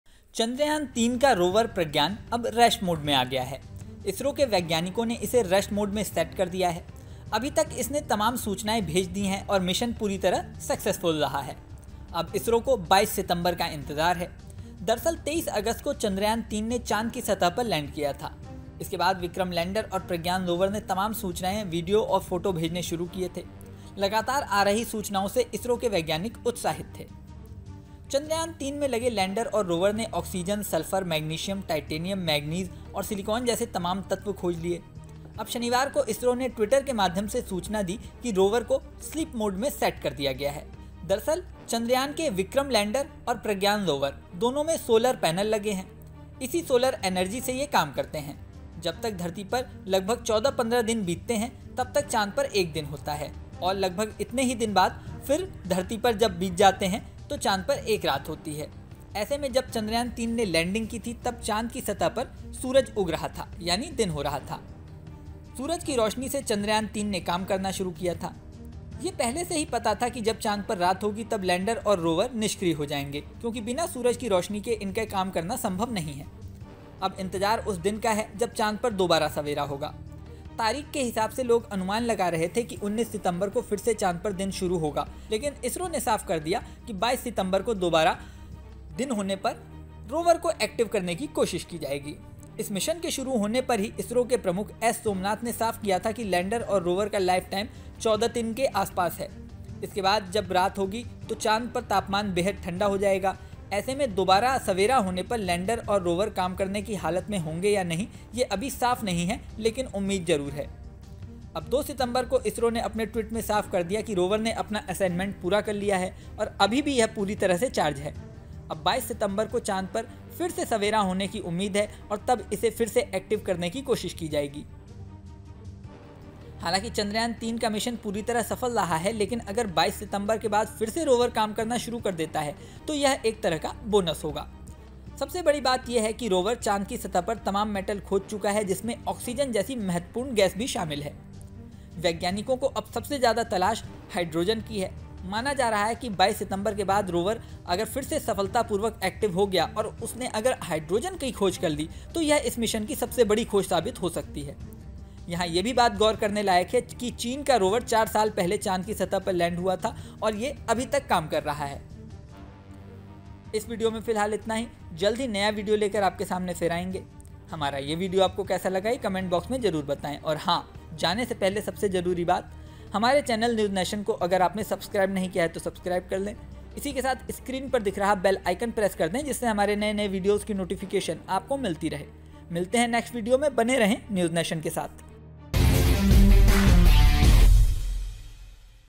Sleeping Mode में गया 'प्रज्ञान', क्या दुबारा चांद पर सवेरा होने पर काम करेगा रोवर?|News State MP-CG
23 अगस्त को चांद की सतह पर सफल लैंडिग करके भारत इतिहास रच चुका है. चांद के साउथ पोल पर उतरने वाला भारत पहला देश बन गया है. वहीं विक्रम लैंडर से बाहर निकला रोवर प्रज्ञान चांद की जमीन पर चहलकदमी करते हुए डेटा इकट्ठा कर रहा था लेकिन अब यह रोवर स्लीप मोड़ पर जा चुका है. जी हां दरअसल रोवर प्रज्ञान को लेकर इसरो का बड़ा अपडेट सामने आया है.

On August 23, India has created history by successfully landing on the surface of the moon. India has become the first country to land on the South Pole of the Moon. At the same time, Rover Pragyan, who came out of Vikram Lander, was collecting data while walking on the moon's land, but now this rover has gone to sleep mode. Yes, in fact, ISRO's big update regarding Rover Pragyan has come out.